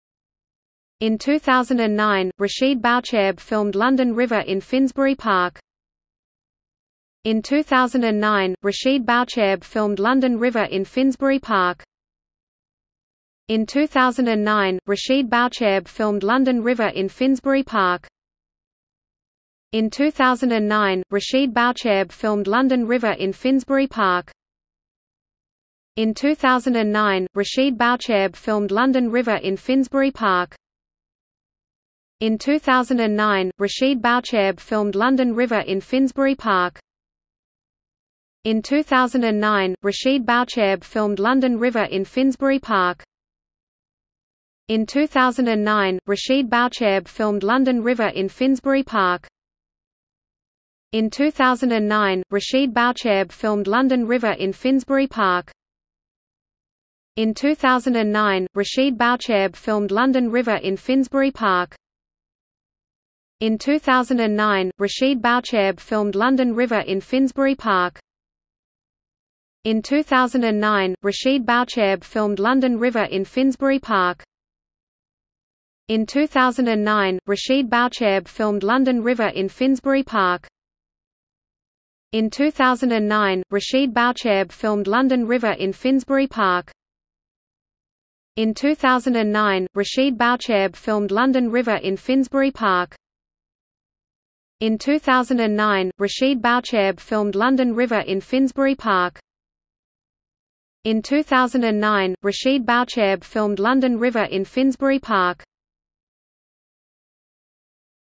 Finsbury Park | Wikipedia audio article
00:00:12 1 Facilities
00:00:24 2 History
00:00:31 2.1 Before the park
00:00:43 2.2 Creation of the park
00:00:56 2.3 During the wars
00:01:08 2.4 Regeneration
00:01:21 3 Live music in Finsbury Park
00:01:33 4 In popular culture

- Socrates

SUMMARY
Finsbury Park is a public park in the London neighbourhood of Harringay. It is in the area formerly covered by the historic parish of Hornsey, succeeded by the Municipal Borough of Hornsey. It was one of the first of the great London parks laid out in the Victorian era. The park borders the residential neighbourhoods of Harringay, Finsbury Park, Stroud Green, and Manor House.